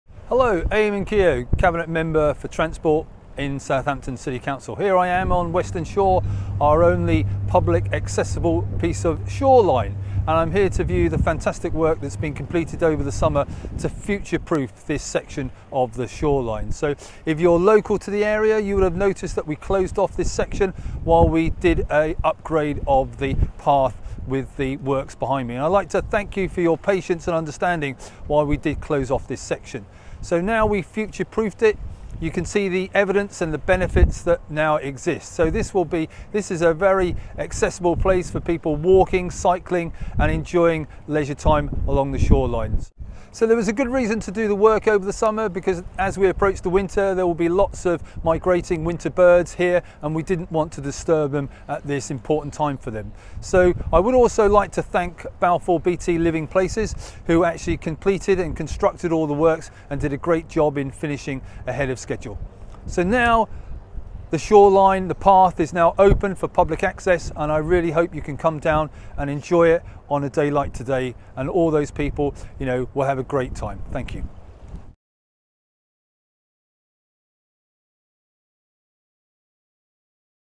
Weston Shore Coastal Erosion Scheme
Councillor Eamonn Keogh talks about the coastal erosion scheme on Weston shore.